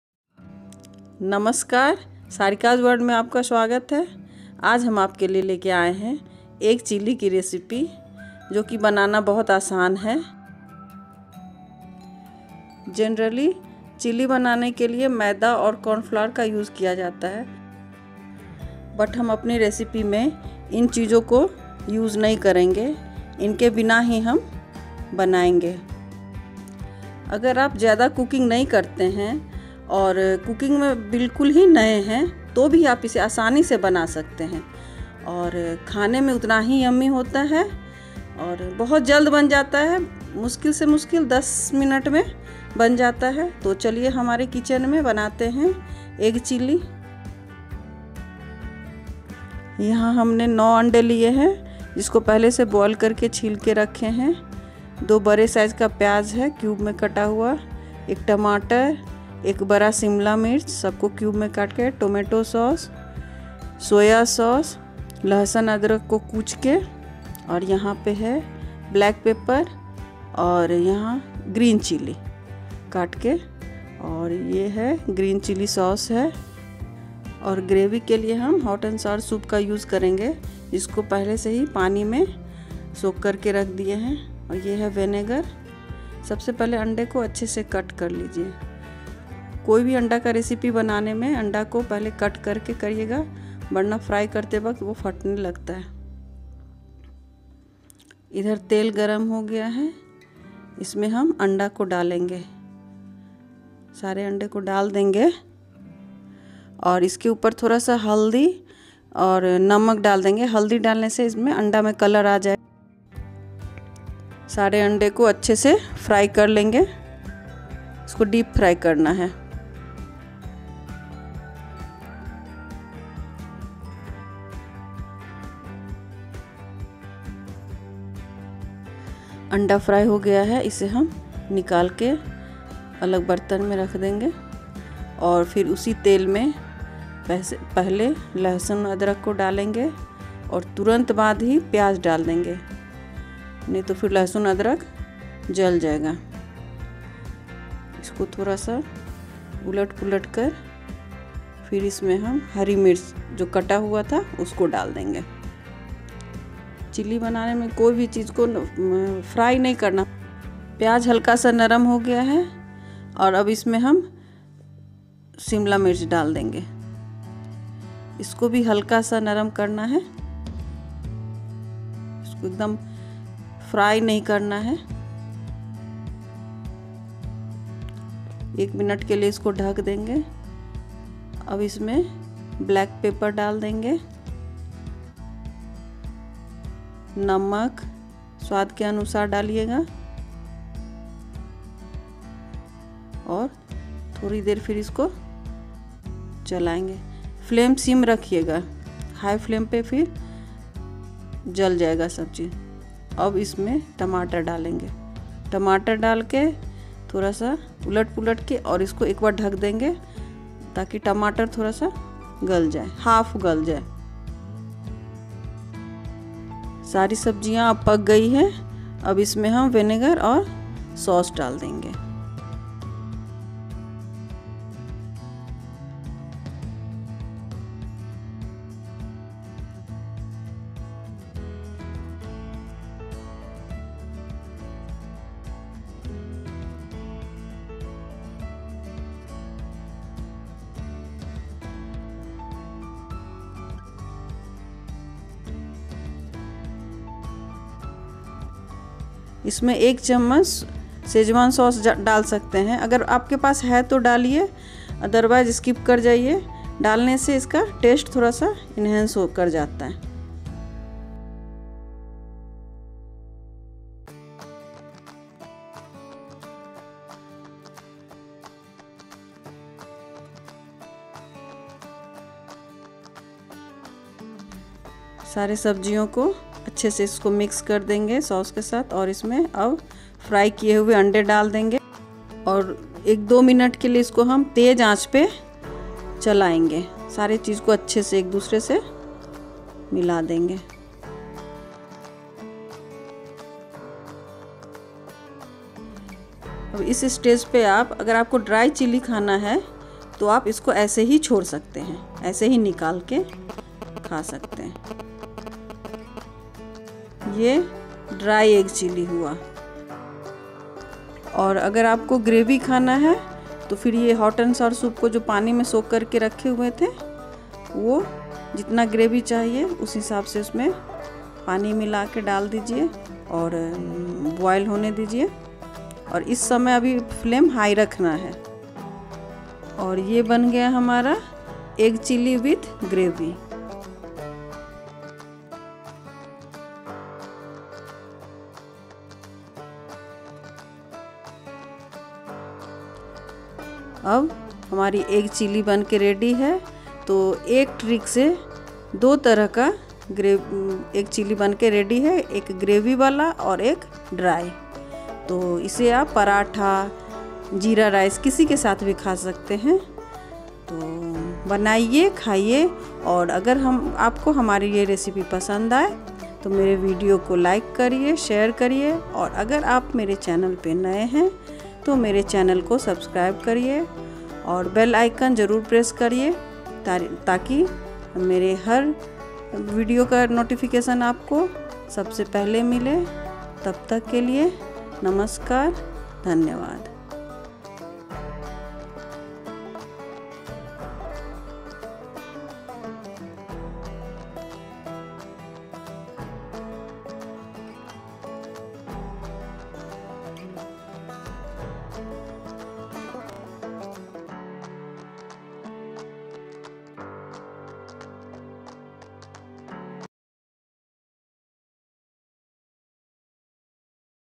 EGG CHILLY IN 2 WAYS || #34
Egg Chilly is a fun dish, packed with flavor. And with a simple trick, you can make your dry egg chilly into a gravy chilly. Perfect when you want a fancy twist to your meals.